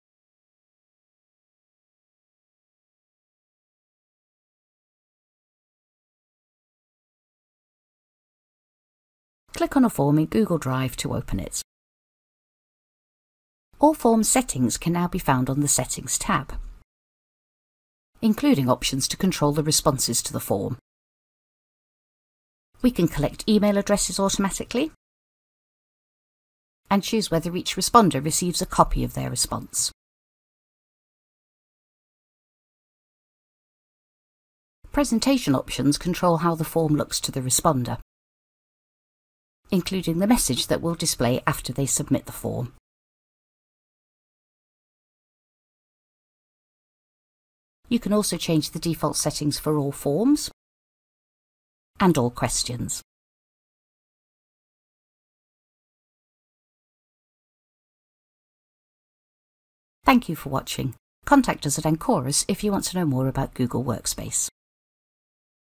Change all Google Form settings in one place
You can now access all Google Form settings via a tab that’s easy to navigate.